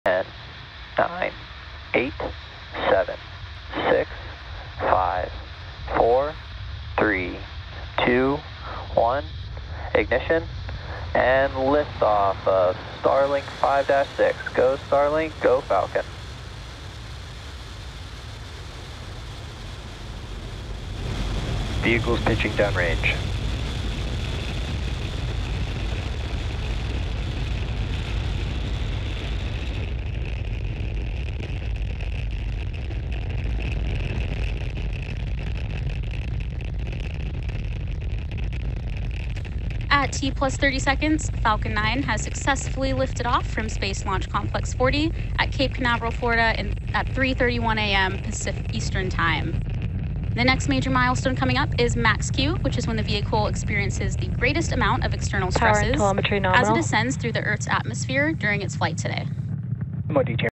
SpaceX Falcon 9 Launch of 56 Starlink Satellites
SpaceX launched a Falcon 9 rocket carrying 56 Starlink satellites to low-Earth orbit on Thursday morning (5/4) at Cape Canaveral Space Force Station in Florida.